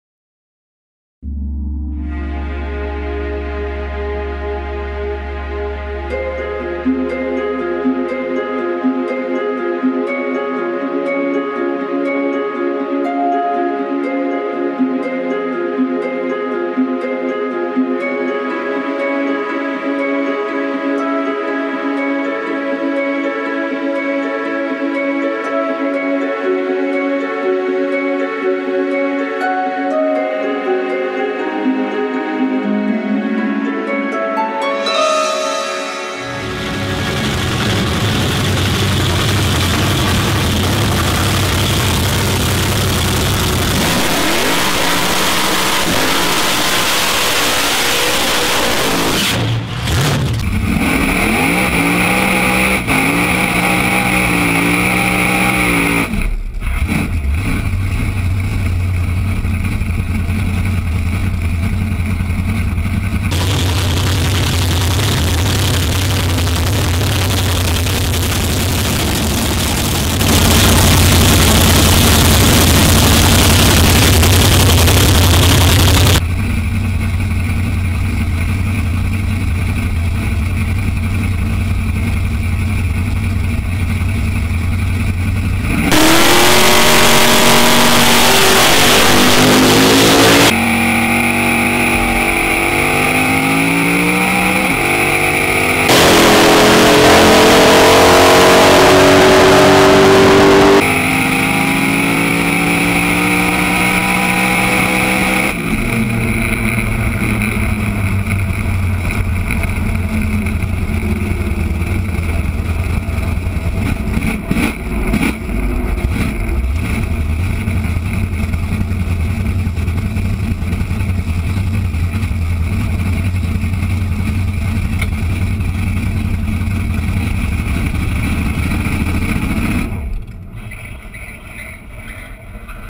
Matt Whiteside Supercharged Outlaws Ute
Check out Matt Whiteside's newly supercharged Holden HR ute at Perth Motorplex. Running low nines due to a misfire (which you can hear a little in the video), this ute is destined for seven second times.
Video taken with Canon 7D, 10-22mm lens and GoPro Hero HD.